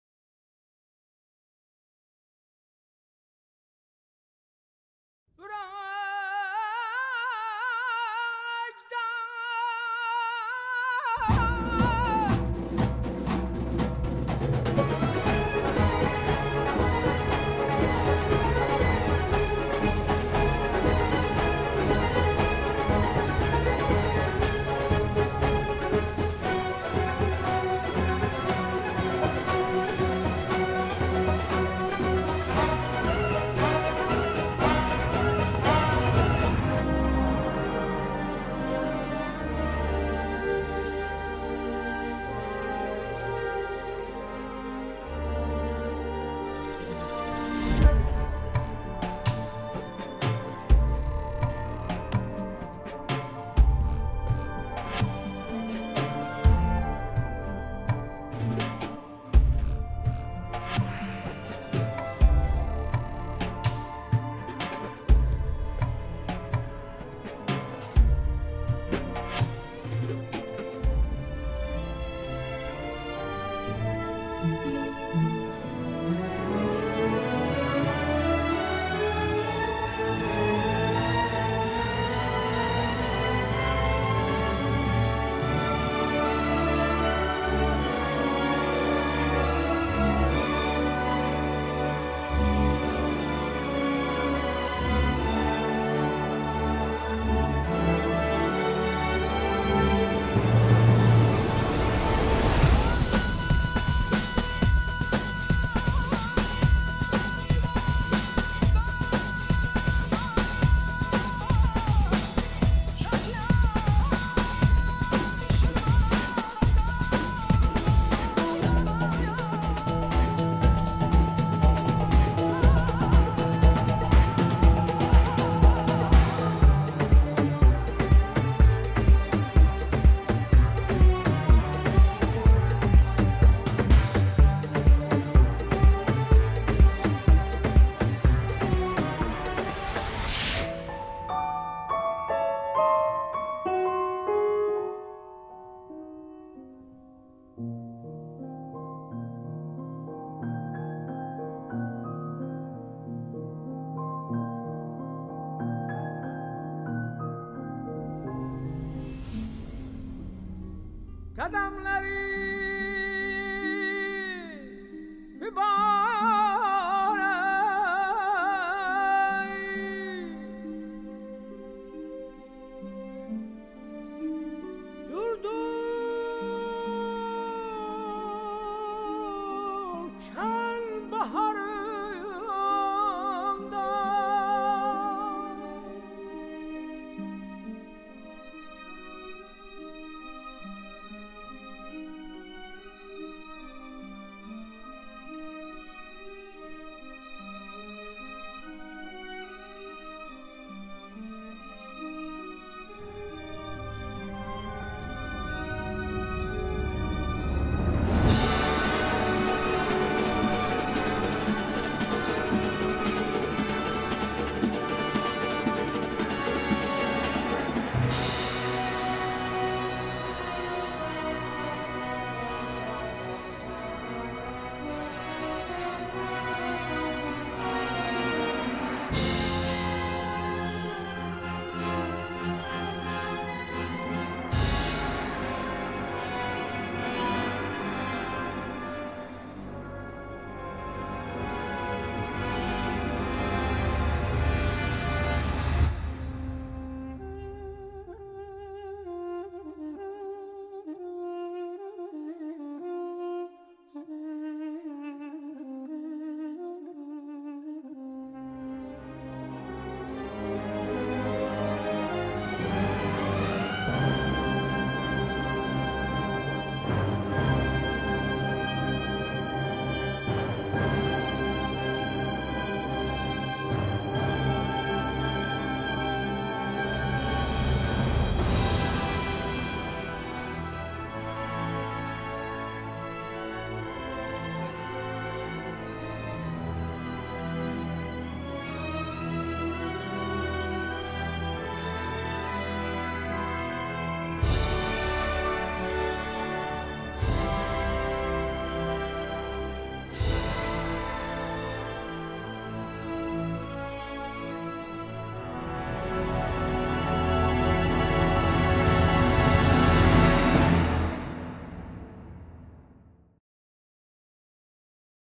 Baku 2015 European Games Opening Ceremony highlights
Relive all the magic of the Baku 2015 Opening Ceremony, featuring Lady Gaga and a cast of thousands and watched live all around the world.